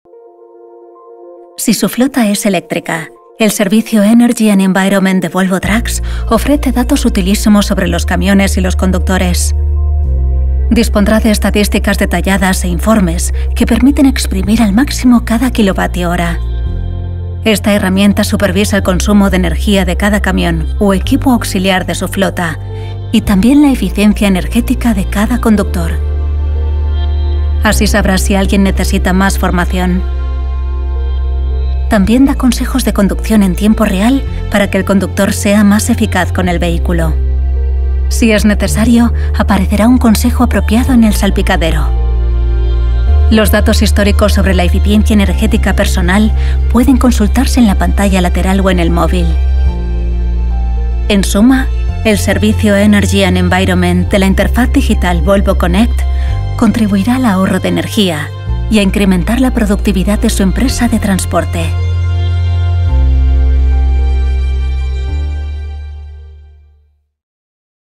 Volvo Trucks: la manera fácil de aumentar la productividad con camiones eléctricos
Cuando opta por la electricidad, el servicio ENERGY AND ENVIRONMENT de Volvo Trucks proporciona información valiosa sobre el rendimiento de sus camiones y conductores. Con estadísticas e informes detallados, aprovechará al máximo cada kilovatio hora. En conjunto, el servicio de Energía y Medio Ambiente, disponible a través de la interfaz digital Volvo Connect, lo ayudará a ahorrar energía y aumentar la productividad de su negocio de transporte.